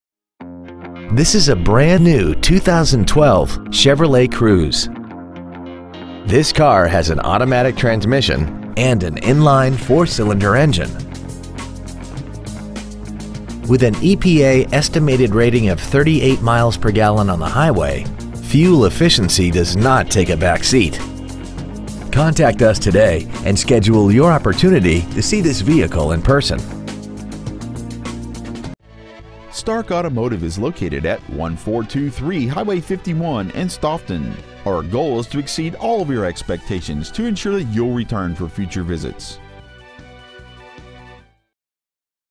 2012 Chevrolet Cruze Stoughton WI
Year : 2012
 Make : Chevrolet
 Model : Cruze
 Engine : 4 cyl., 1.4, sequential-port f.i.
 Trans . : automatic
 Exterior : summit white

 Interior : jet blacksport red
 Stock : 6120904